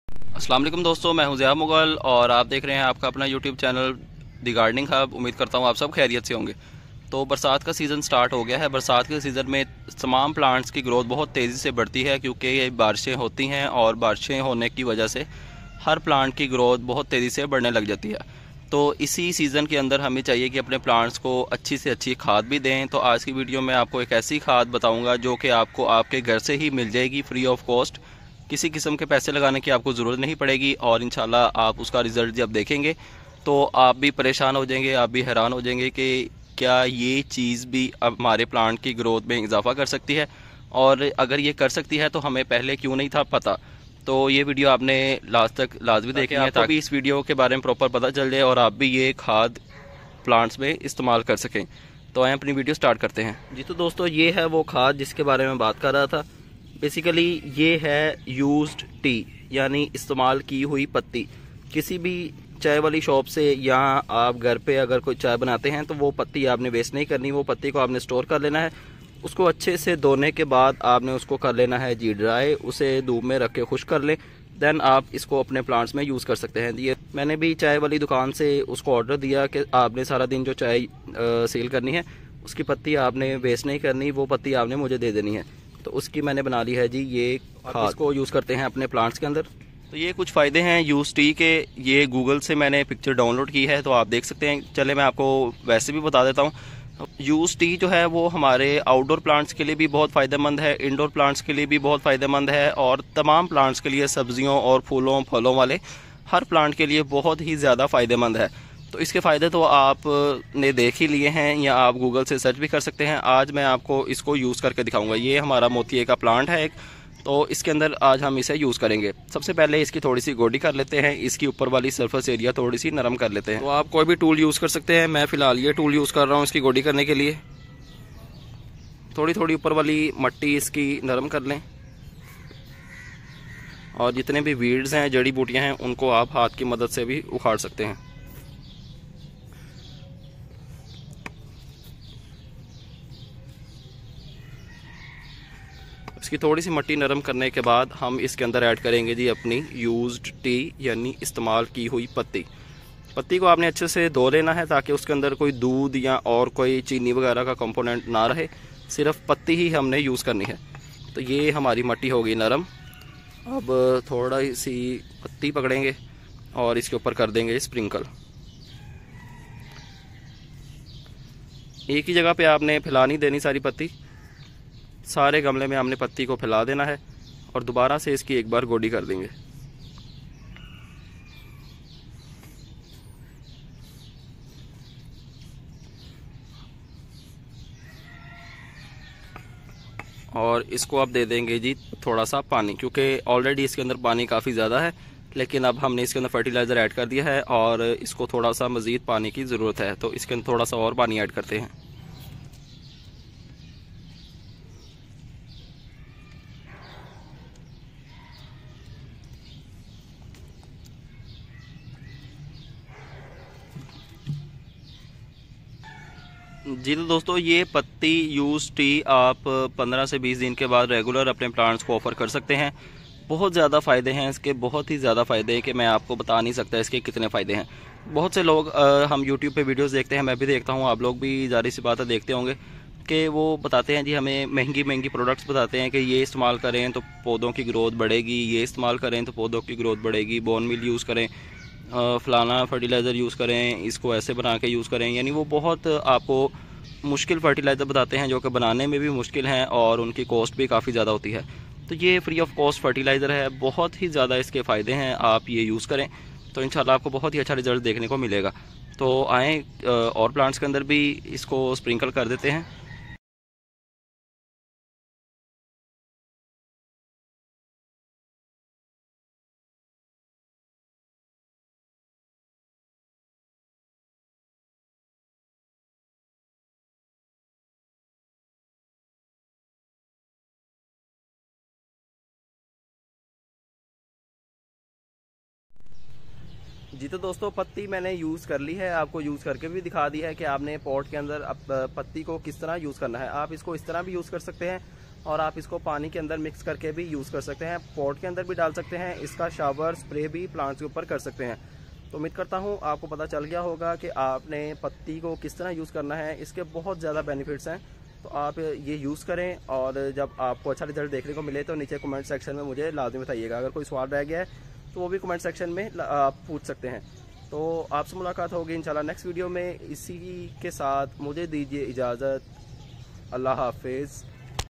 Best Organic Fertilizer For All Plants. Tea Fertilizer. Podhon K Liye Organic Khaad Bnany Ka tareka.
CHANNEL CREATED BY ZIA MUGHAL

►ASSALAM O ALAIKUM ALL,  Best Organic Fertilizer For All Plants. Tea Fertilizer. Podhon K Liye Organic Khaad Bnany Ka tareka.
YOU WILL NEVER THROW AWAY USED TEA AFTER WATCHING THIS. Used Tea Is Best Fertilizer For All Plants.